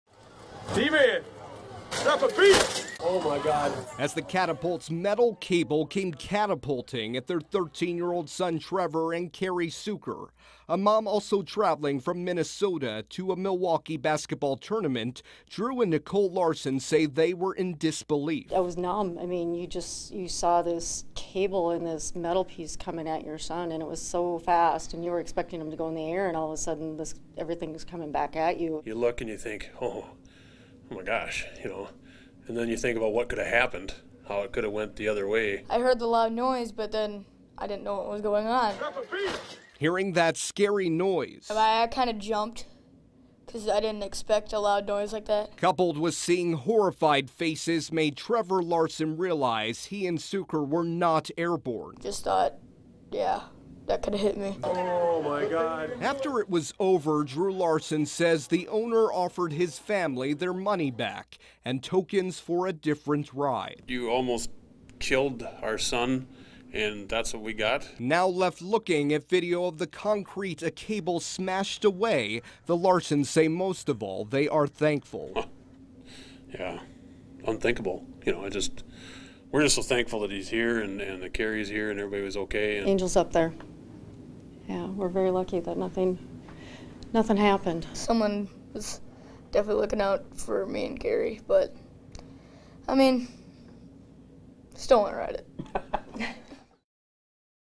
Thrill ride cable snaps just before launch
A metal cable on a catapult ride in Wisconsin Dells, Wisconsin, snapped just before the ride was supposed to launch. No one was hurt in the incident, which a passenger's father caught on video.